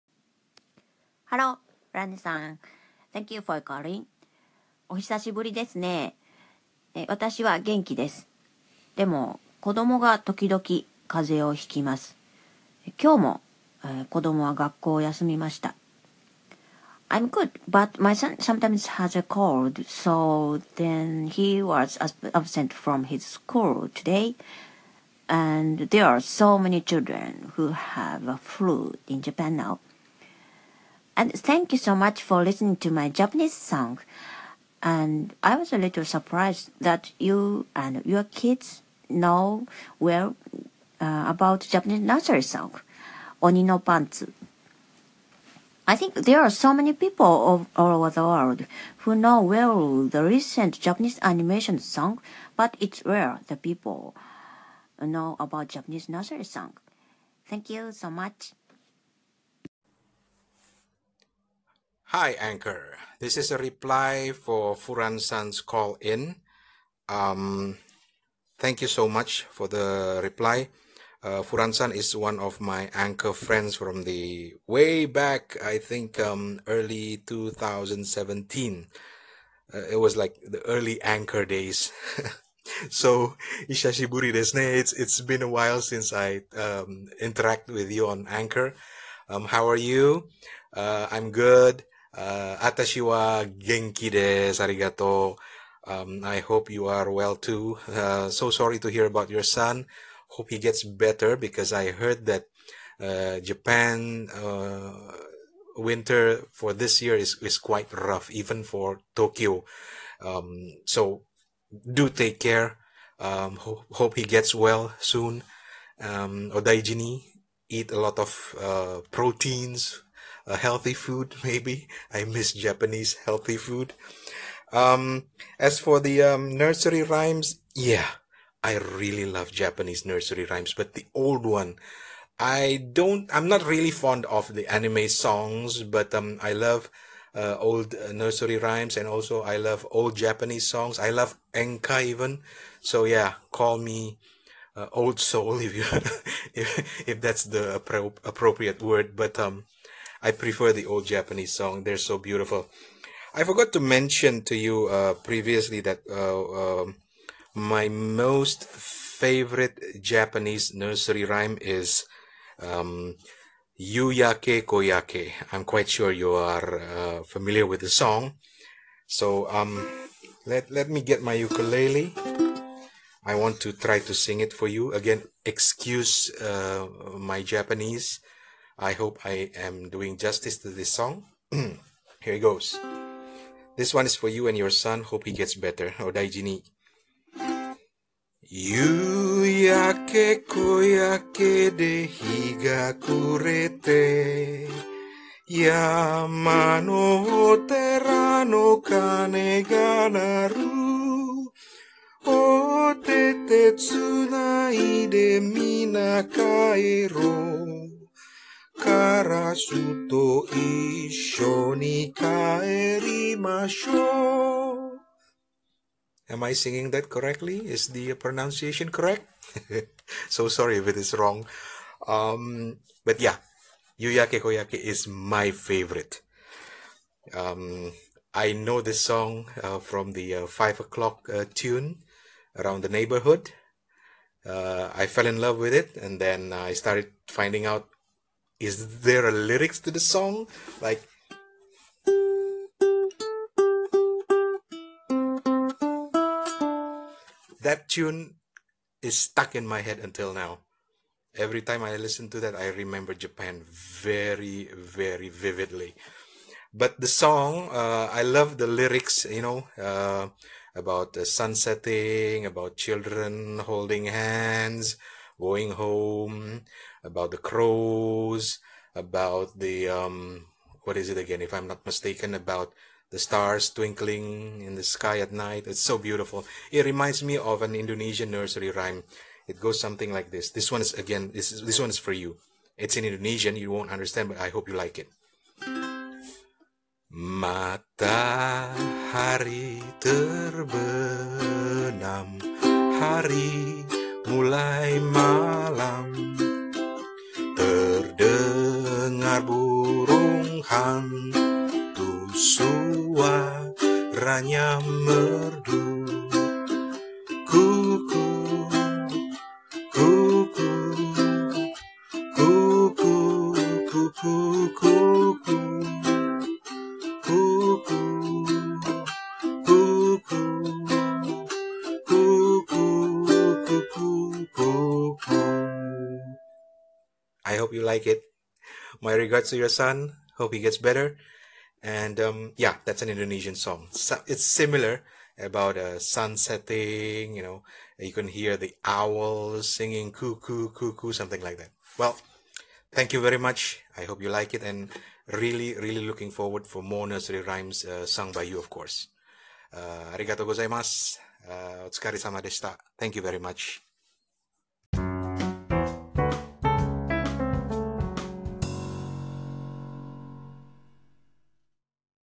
Nursery Rhymes
A reply to Fu-ran San about Japanese and Indonesian Nursery Rhymes. Arigatou Fu-ran San 😊